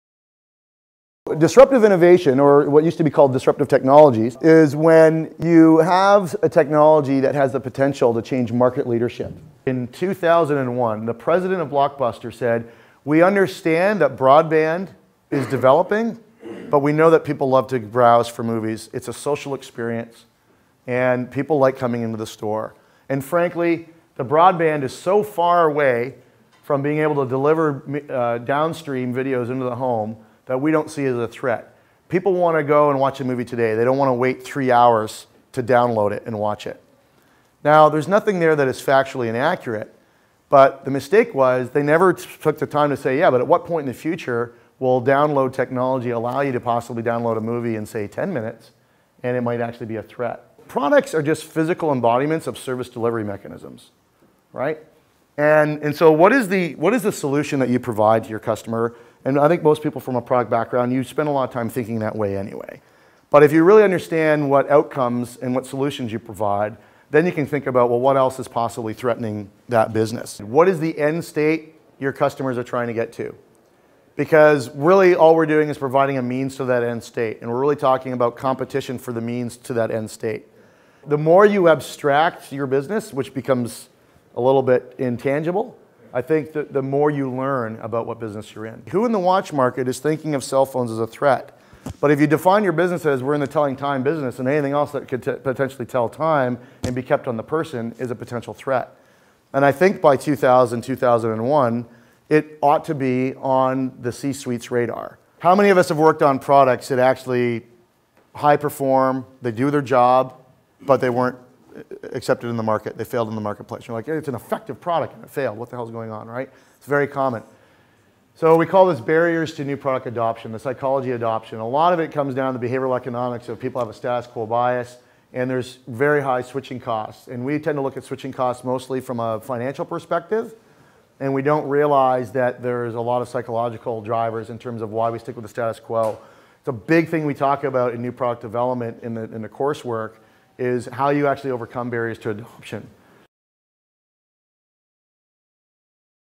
Inside Sauder Executive Education: Disruptive Innovation | Sauder School of Business, UBC, Vancouver
Professor Tim Silk discusses distruptive innovation, when you have a technology with potential to change market leadership.
For start dates, registration and to find out more about Sauder Executive Education's Product Management Bootcamp,